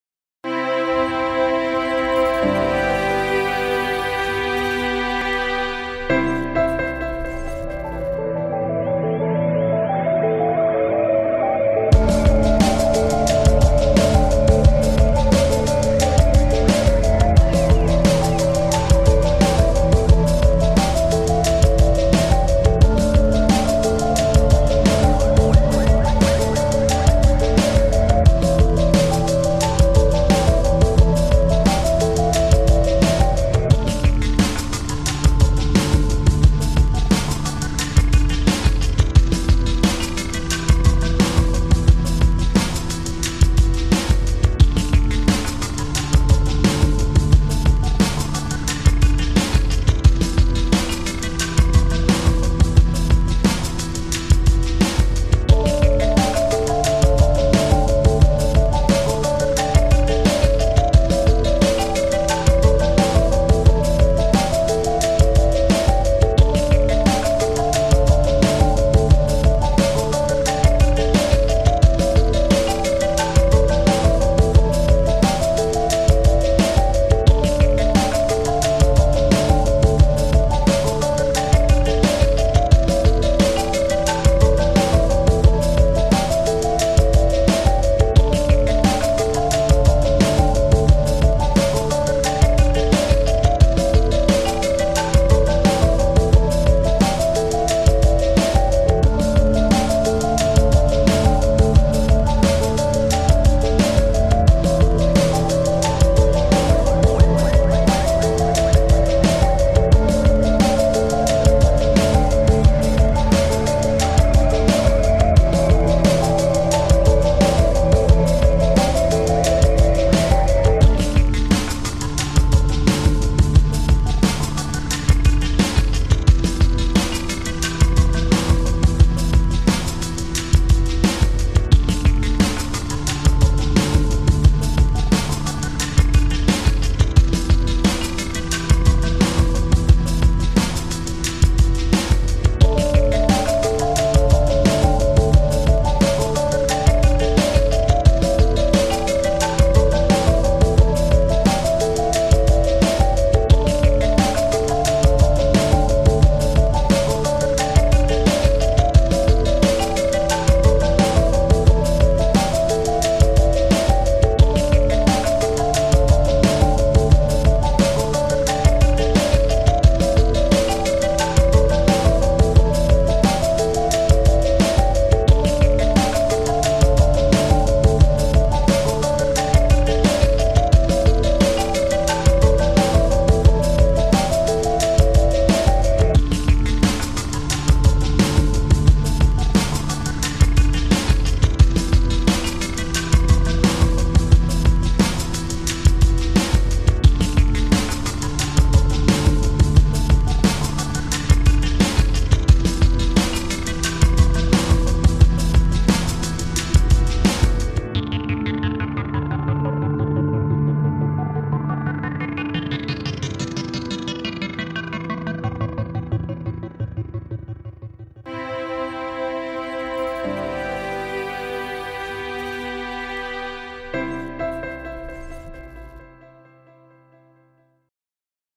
Cellulose potential unveiled, INACELL by TECNALIA
INACELL Technology produces strong fiber-fiber bonds.